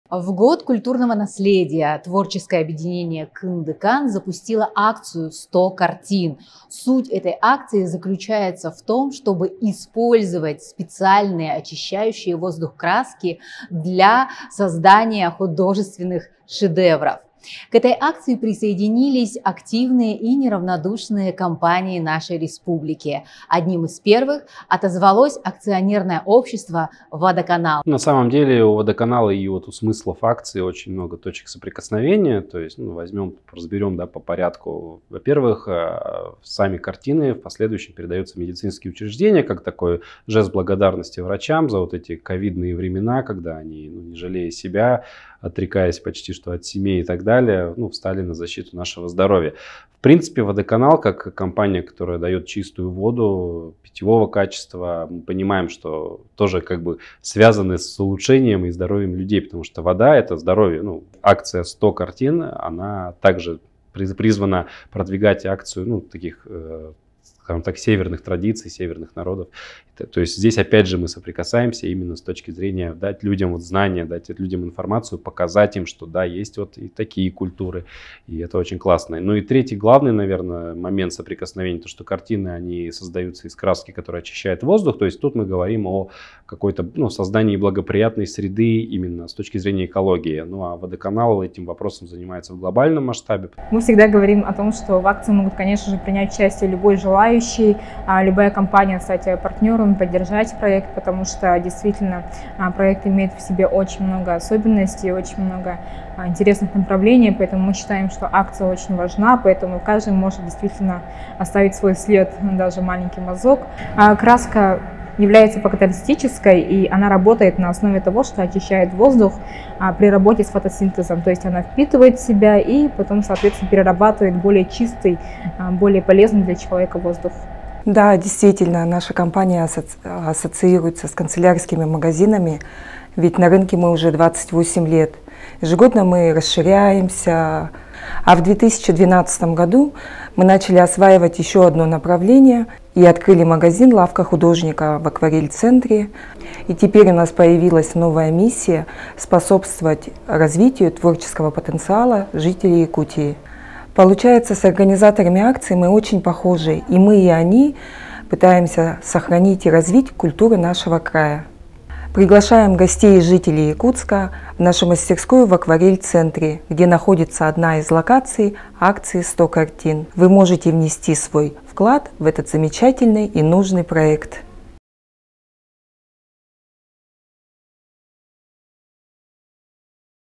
Акция "100 картин мед учреждениям Якутии" 1 СЮЖЕТ
С 14 декабря 2022 г по 19 января 2023 года Творческое объединение «Кындыкан» при поддержке Генерального Партнера АО «ВОДОКАНАЛ» и Министерства по внешним связям и делам народов РС(Я) проводит акцию «100 картин».

Таким жестом мы привлекаем внимание большинства к значимым вопросам Севера, а также выражаем благодарность медицинским учреждениям Республики, передавая им уникальные картины, нарисованные краской со свойством очистки воздуха.

Каждый из вас может стань частью большой истории!

Для популяризации акции «100 картин» проводится большой новогодний розыгрыш призов для всех желающих!

Победители будут определяться путем рандома (специальное приложение) каждую неделю по средам с 21 декабря 2022 по 18 января 2023 г. в Telegram-канале Kyndykan.

Подробнее об условиях участия читайте в Положении о конкурсе в рамках акции «100 картин» на сайте KYNDYKAN.COM.

Партнеры акции уже оставили свой след в акции «100 картин», успей и ТЫ!
Приходи в креативную студию «АйАрт» по адресу: ул. Пояркова 5, 2 этаж (вход через магазин «Лавка художника») в будние дни с 12:00 до 17:00, а также в любую кофейню «Bon Ami» и рисуй!

Желаем удачи!

Новогоднее волшебство уже рядом!